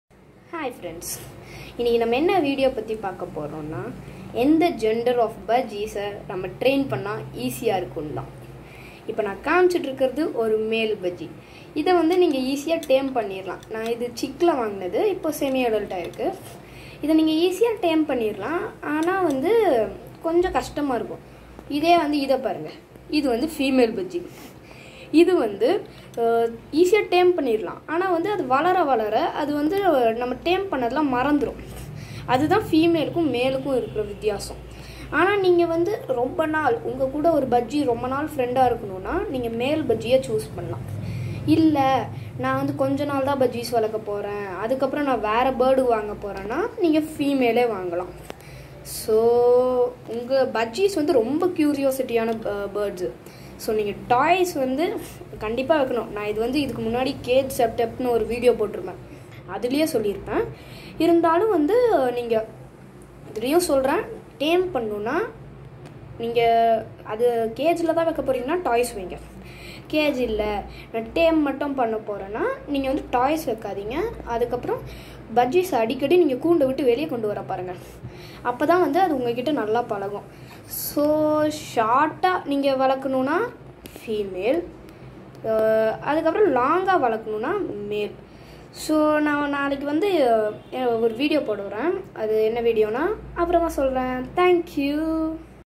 Easily tameable budgie gender
This video is about easily tameable gender of budgies...